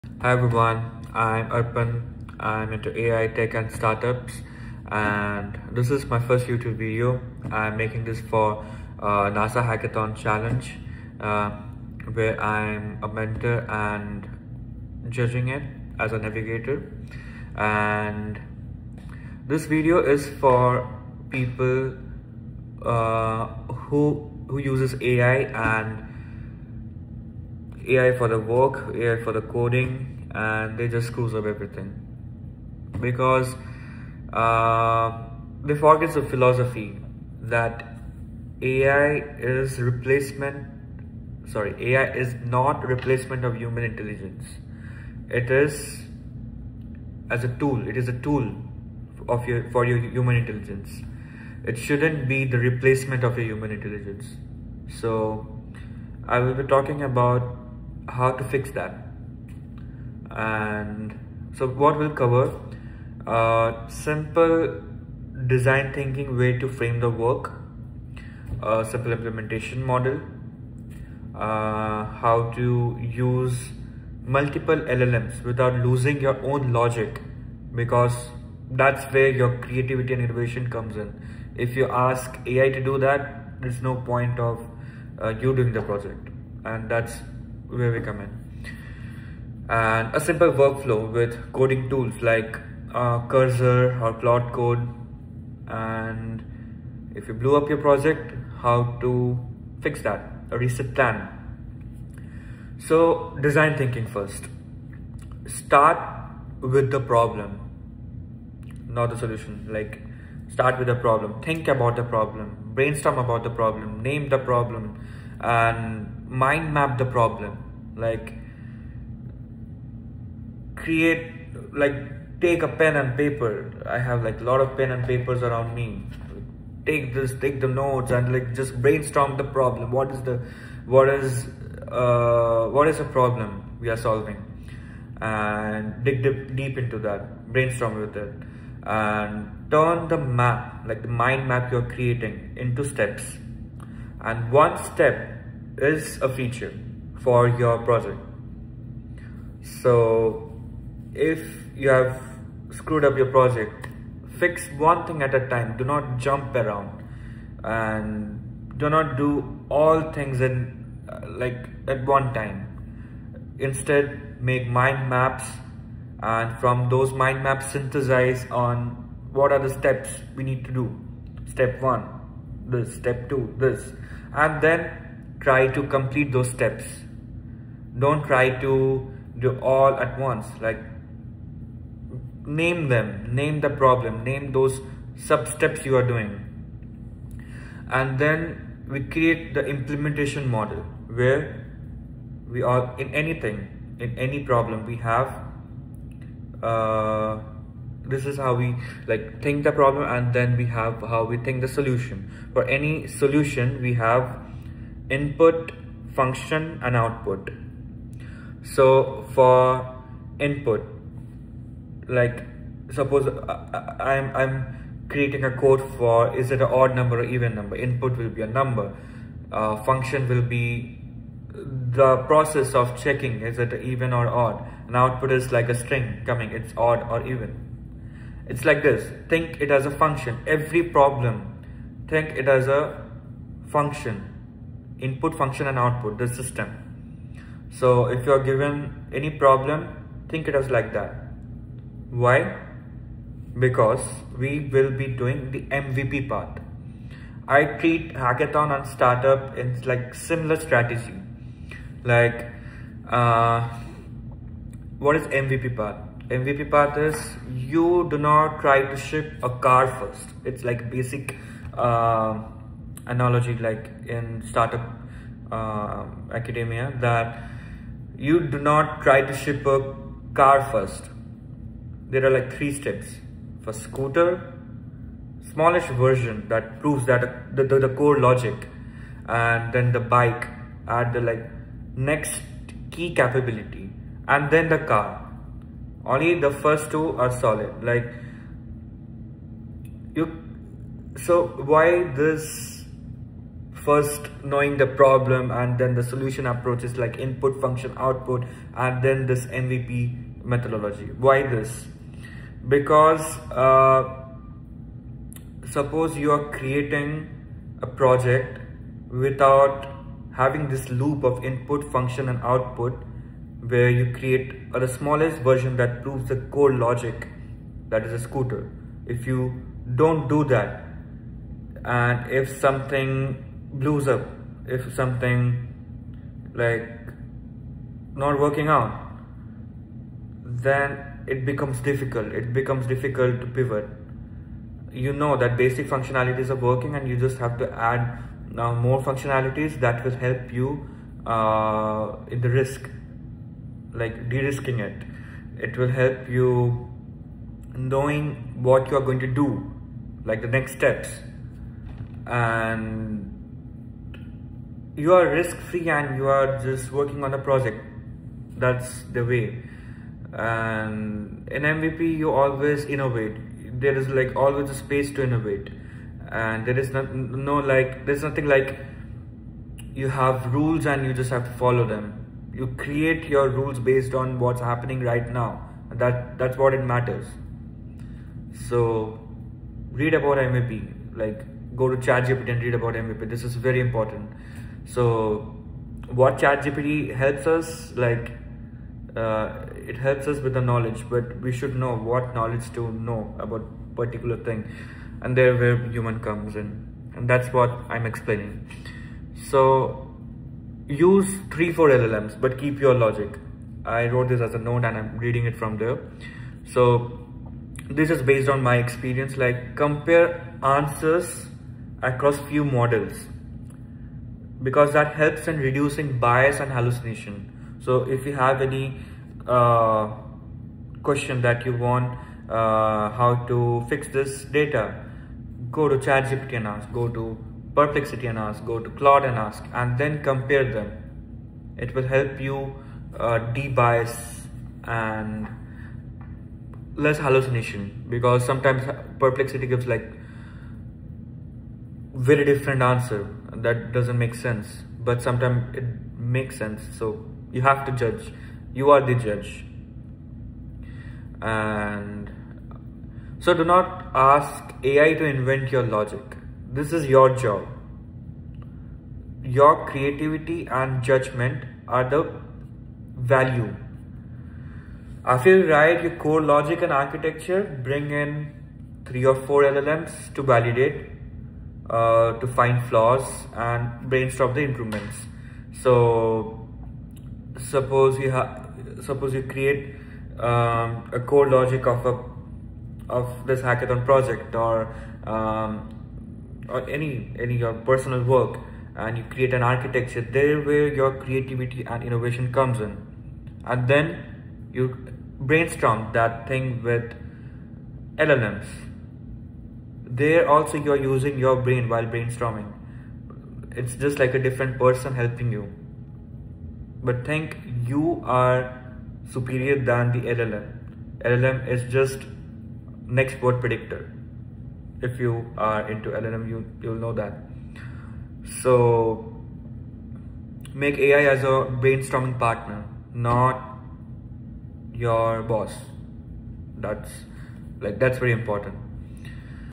Don’t replace human intelligence with AI. Use it as a tool.
Hey everyone, I’m Arpan. This is my first YouTube video. I made it for NASA Space Apps 2025, and it works for any hackathon and any startup team. Startups are a hackathon every day.

If AI wrecked your code and you tried to fix it with more AI, if hallucinations are haunting your project, this is for you. The core idea is simple. Do not replace human intelligence with AI. Make AI a tool. You are the thinker and the decider. AI is the power drill you pick up when it is time to build.

• A design-thinking way to frame the work
• A simple Input → Function → Output contract for every function
• MVP path: scooter → bike → car
• How to use 3–4 LLMs without losing your own logic
• A safe workflow with Cursor or Claude Code using a control file
• A tight loop to ship one subfeature at a time
• A clean restart plan if your repo already blew up

• You lead. AI assists.
• Contracts make coding a translation job, not guesswork.
• Small steps reduce risk and keep momentum.
• Multiple LLMs help you review without inventing your logic.
• A control file keeps tools aligned with your plan.

Chapters
00:00 Intro
00:20 Who this is for
00:33 Core philosophy
01:43 Design thinking, fast
03:25 Input → Function → Output
04:00 MVP: scooter → bike → car
07:48 Use 3–4 LLMs without losing logic

Ask me anything: Book a 15 minute slot